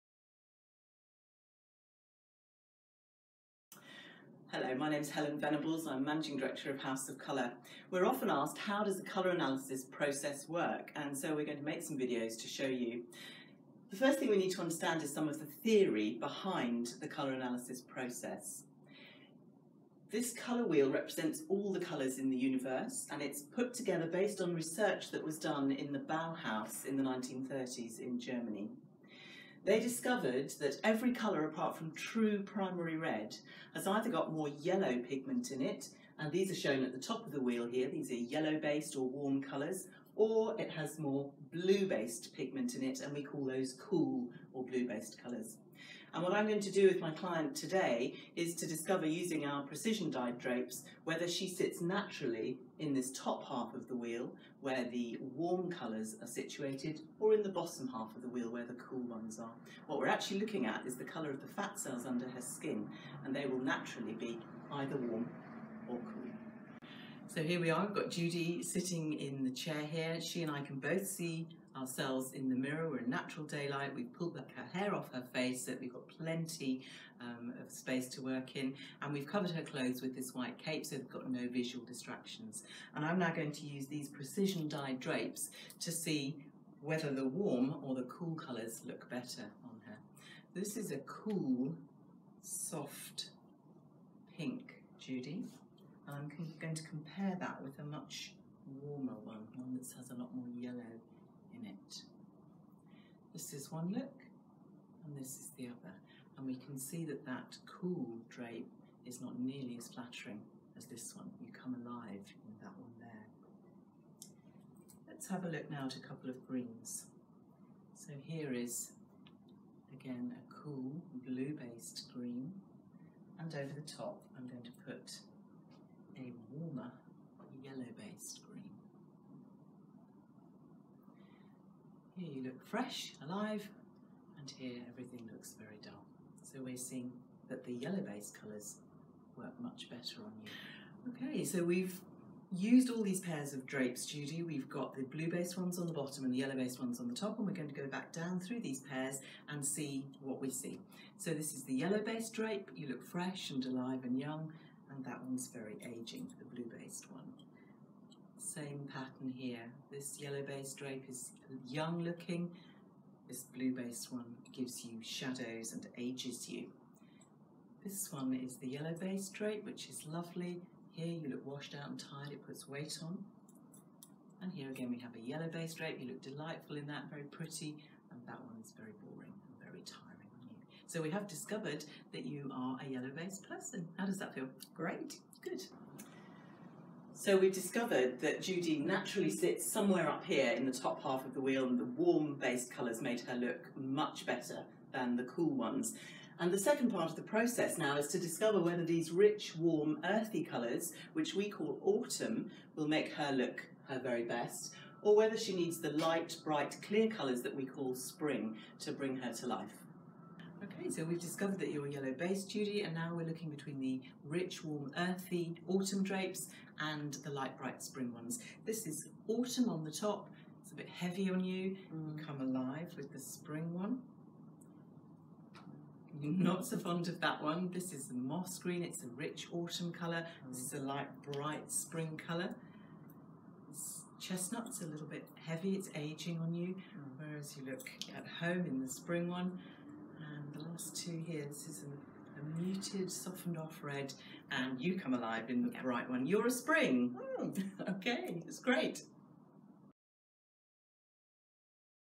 The colour analysis process 1 of 3
How does the Colour Analysis process work with House of Colour?